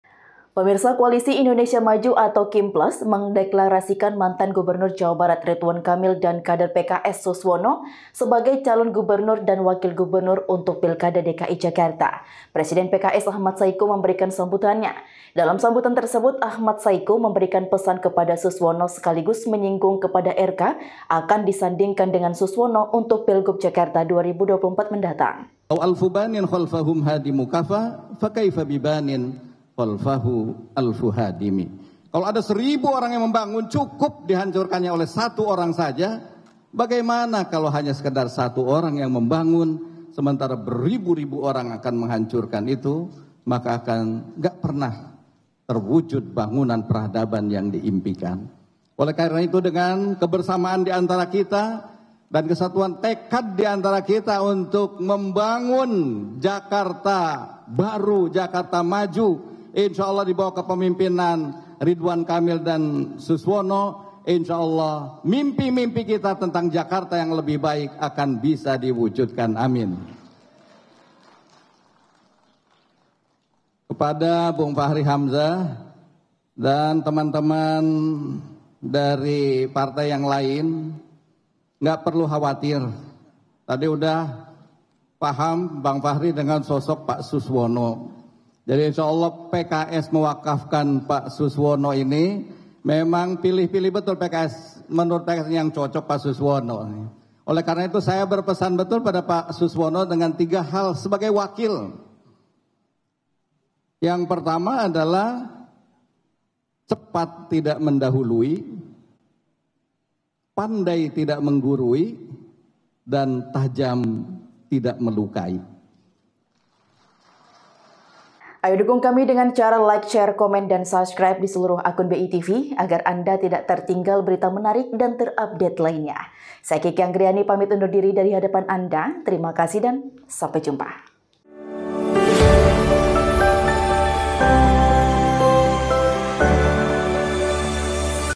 Presiden PKS Ahmad Syaikhu Sampaikan Pesan hingga Harapan di Deklarasi RK-Suswono
Koalisi Indonesia Maju (KIM) Plus mendeklarasikan mantan Gubernur Jawa Barat Ridwan Kamil (RK) dan kader PKS, Suswono, sebagai calon gubernur dan wakil gubernur untuk Pilkada DKI Jakarta 2024. Presiden PKS, Ahmad Syaikhu memberikan sambutannya. Dalam sambutan tersebut, Ahmad Syaikhu memberikan pesan kepada Suswono sekaligus menyinggung kenapa RK akan disandingkan dengan Suswono untuk Pilgub Jakarta 2024 mendatang.